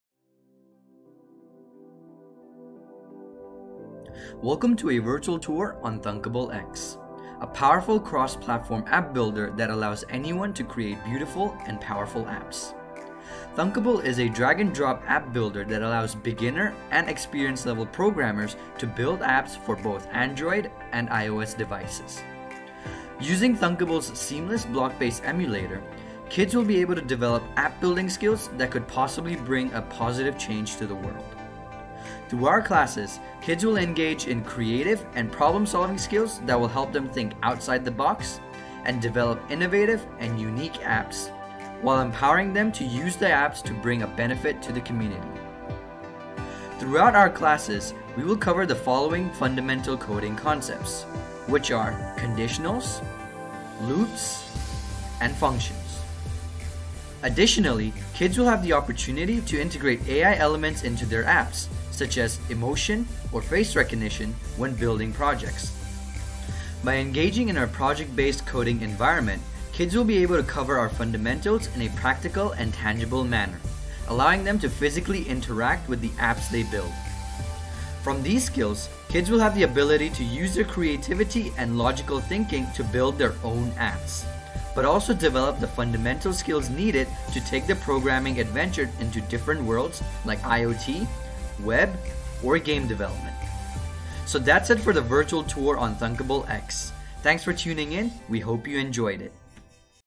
Cobo Thunkable Virtual Tour
Thunkable enables anyone to create beautiful and powerful mobile apps that run on Android and iOS devices. Listen in to see what your child can learn and build in our Thunkable course.